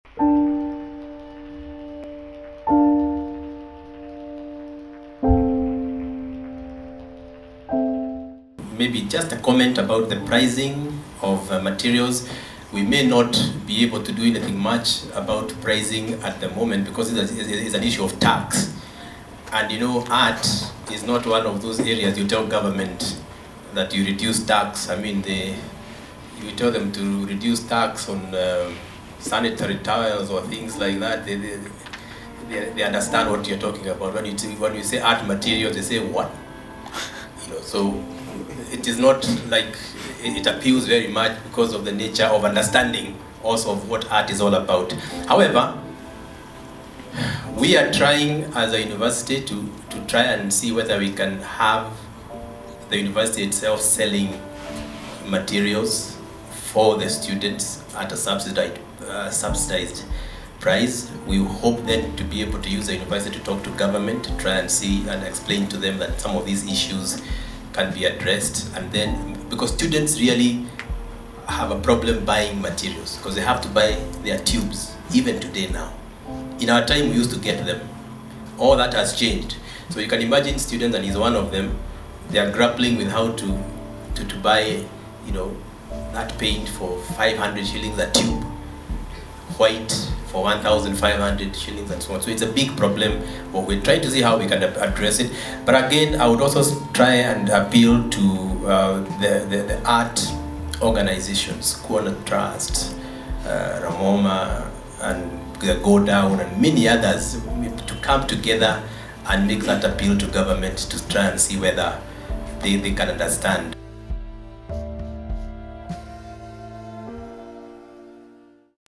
Kenya Art Fair 2014 - Art and Education
Rr. Kamau - Kenyatta University on Art and Education